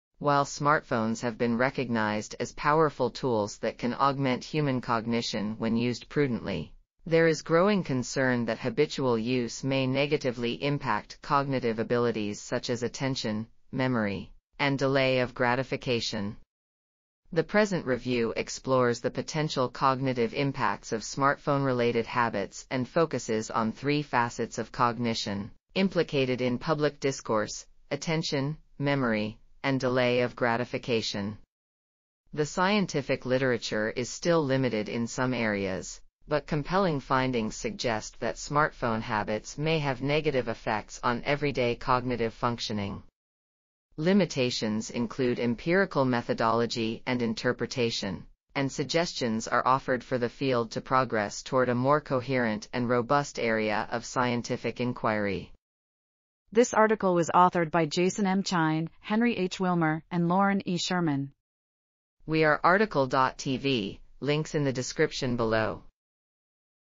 Smartphones and Cognition: A Review of Research Exploring the Links between Mobile Te... | RTCL.TV
### Keywords ###

### Article Attribution ###
Title: Smartphones and Cognition: A Review of Research Exploring the Links between Mobile Technology Habits and Cognitive Functioning
Authors: Jason M. Chein, Henry H. Wilmer ,and Lauren E. Sherman
Publisher: Frontiers Media S.A.

### Video Timestamps ###
0:00:00 - Summary
0:00:56 - Title
0:01:02 - Tail
0:01:06 - End